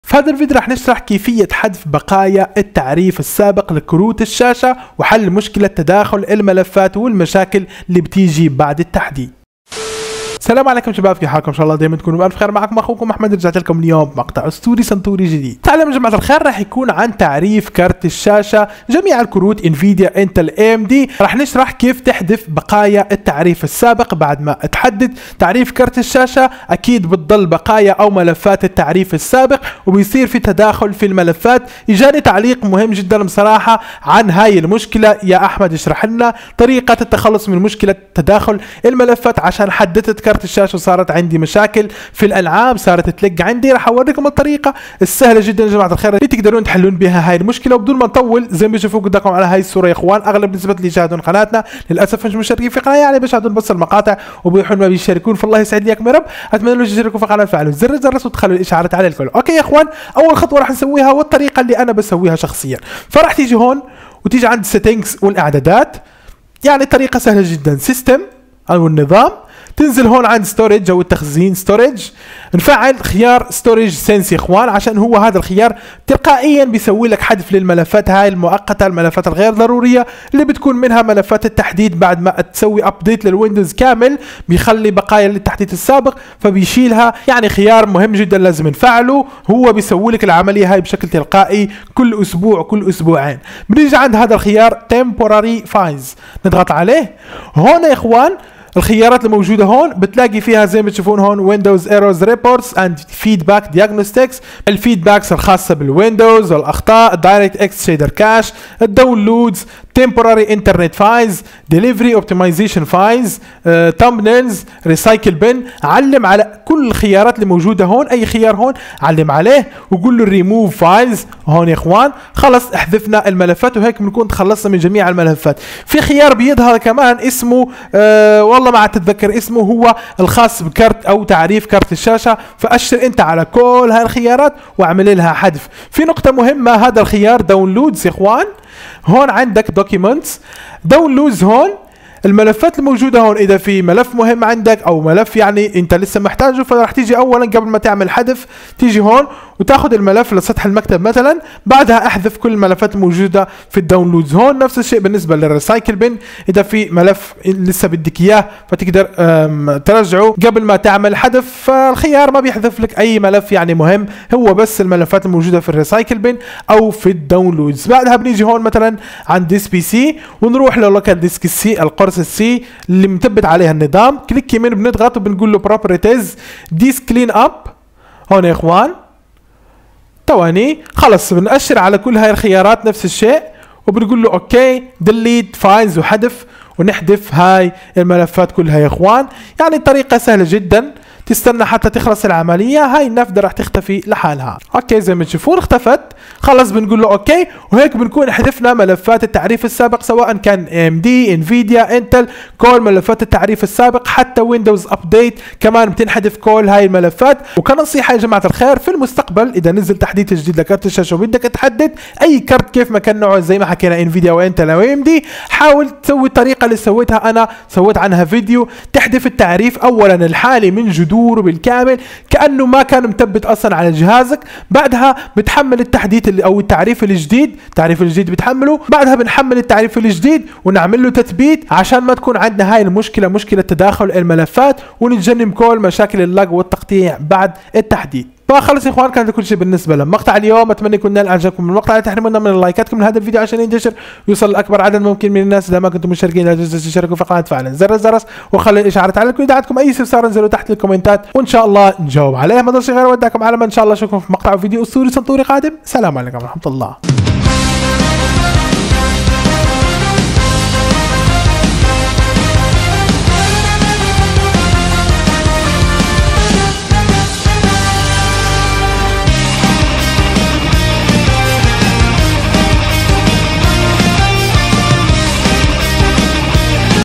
طريقة حذف بقايا التعريف القديم لجميع كروت الشاشة 👍🛠
يعتبر تحديث بطاقة الرسومات وتعريف الشاشة واحدًا من أهم الخطوات التي يمكنك اتخاذها لتحسين أداء جهاز الكمبيوتر الخاص بك. ومع ذلك، قد يتسبب تثبيت تعريف جديد لكرت الشاشة في وجود بقايا من التعريف القديم يمكن أن تتسبب في مشاكل واضطرابات في النظام. لحذف بقايا التعريف القديم بنجاح، يجب اتباع الخطوات الصحيحة. في هذا الفيديو، سنشرح كيفية حذف بقايا التعريف القديم لكرت الشاشة بسهولة.

مع تطور التكنولوجيا وتقدم أجهزة الكمبيوتر وبطاقات الرسومات، أصبح من الضروري البحث المستمر عن أحدث التعريفات والبرامج لضمان أداء ممتاز وأفضل تجربة مستخدم. واحدة من أهم مكونات أي نظام كمبيوتر هي كرت الشاشة، الذي يؤثر بشكل كبير على جودة الرسوميات وأداء الألعاب والتطبيقات. لكن عند تثبيت تعريف جديد لكرت الشاشة، يمكن أن تظل بقايا التعريف القديم تتسبب في مشاكل واضطرابات في النظام. لذلك، سنستعرض في هذا الفيديو كيفية حذف بقايا التعريف القديم لكرت الشاشة بشكل صحيح، وذلك لضمان أن نظام الكمبيوتر الخاص بك يعمل بكفاءة ويستفيد من أحدث التحسينات التي تقدمها الشركات المصنعة لبطاقات الرسومات.